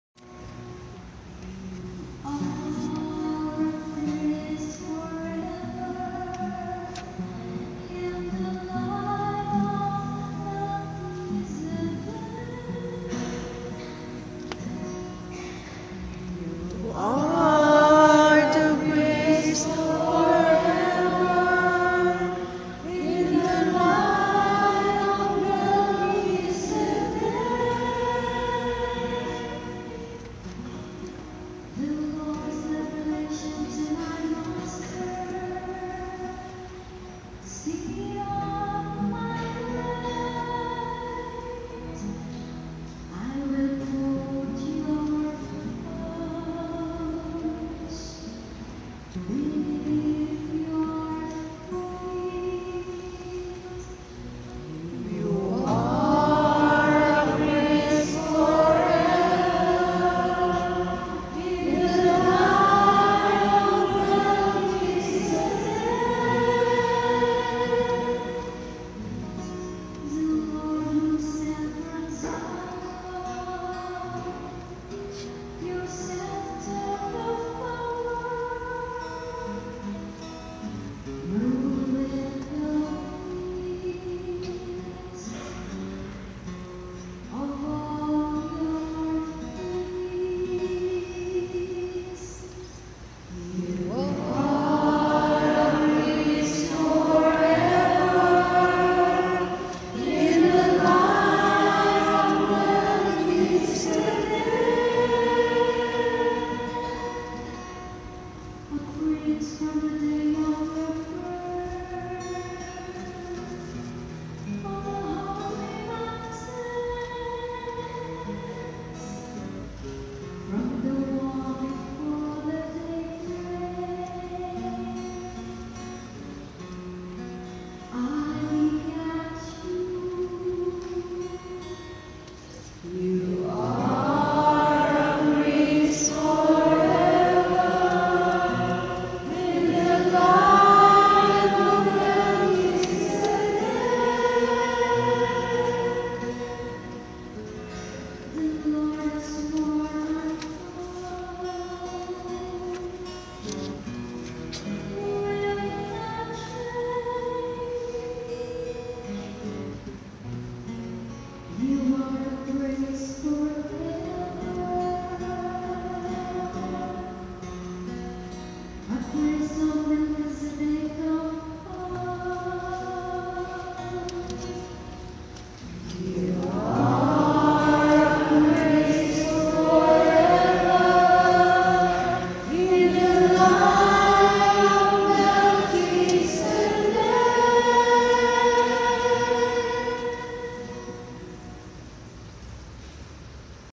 Beautiful Angelic Voice singing at the Church.
Jos Garcia while singing Responsorial psalm at Kyoto Catholic Church. June 23 2019.

Video owner; Jos Garcia